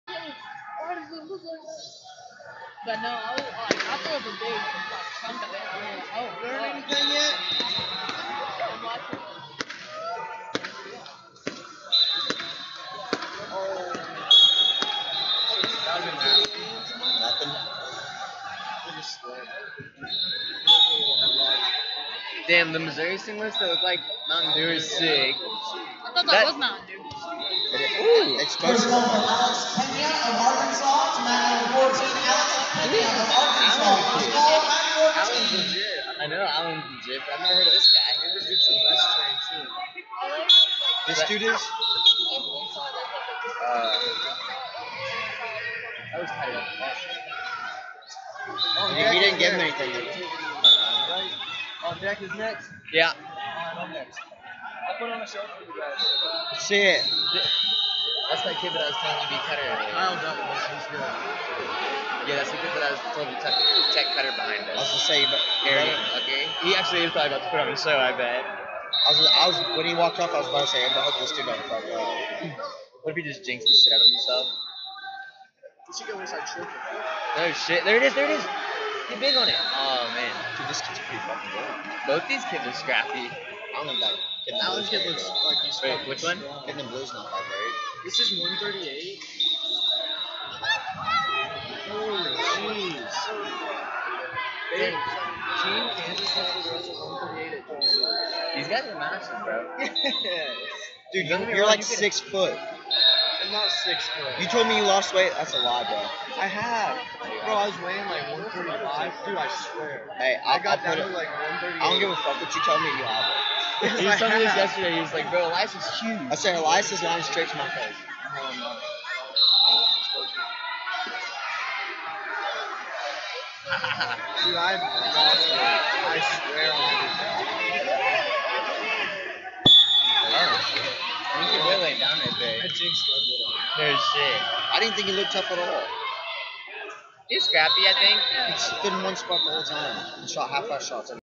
Junior Boys 138 Kade Moore Best Trained Wrestling Vs Kage Lenger Victory Wrestling
2021 Southern Regionals - Junior Boys 138 Kade Moore Best Trained Wrestling Vs Kage Lenger Victory Wrestling